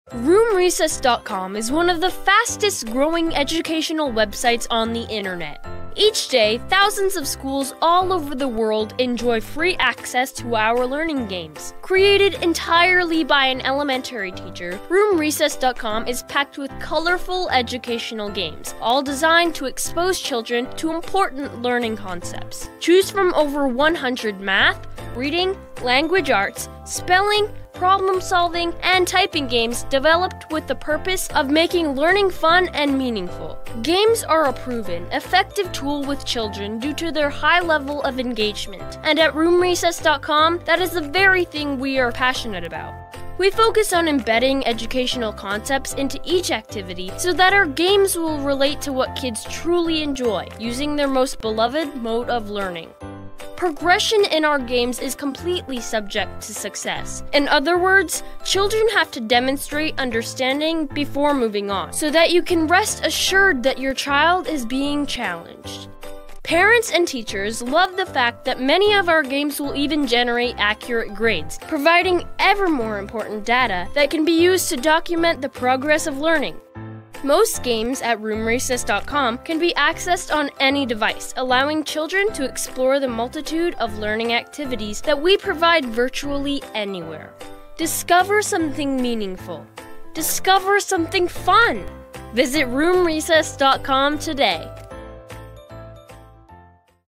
RoomRecess.com 2018
RoomRecess.com offers over 100 learning games in subjects such as math, reading, language arts, spelling, problem solving and even typing. Most of our games are playable on any device.